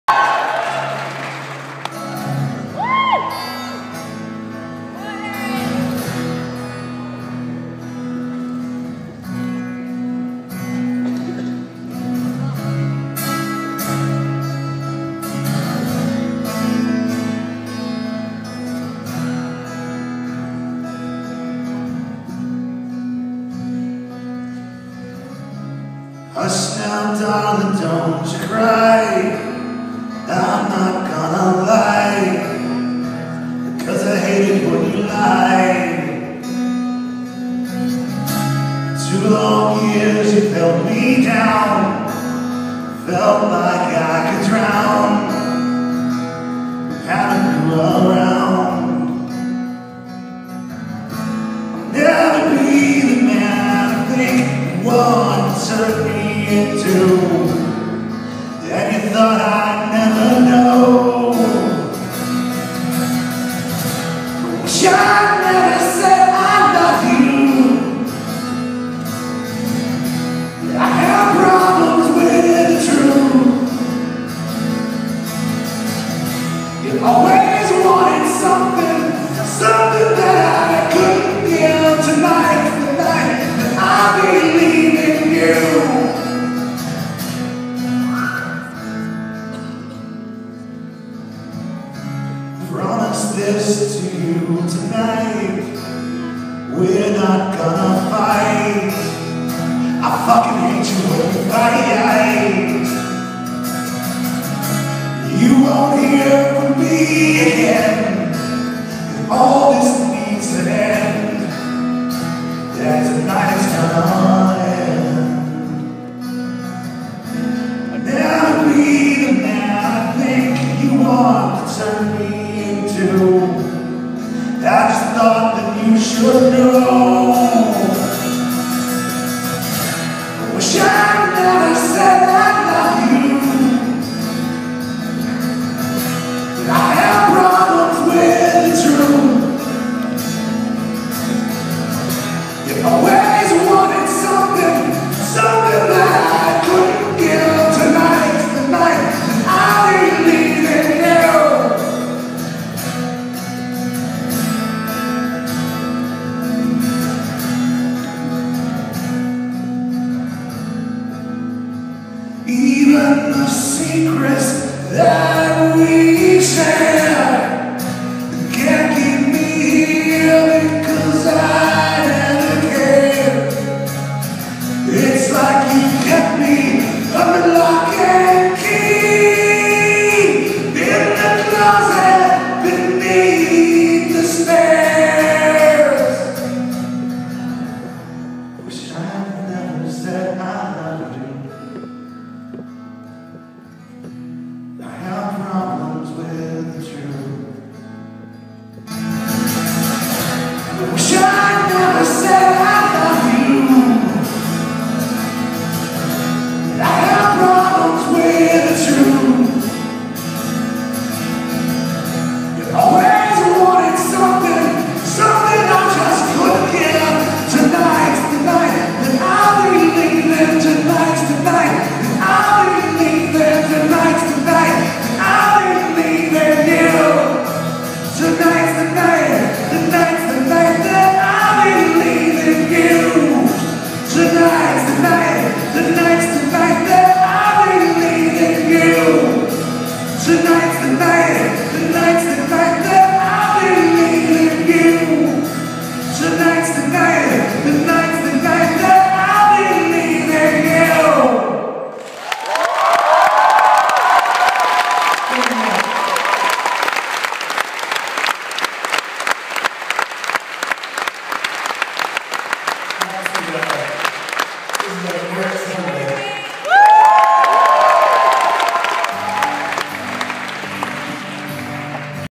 Bowling For Soup Acoustic Night Erik Chandler Solo Song 9/10/13 LIVE Farewell UK tour
at union chapel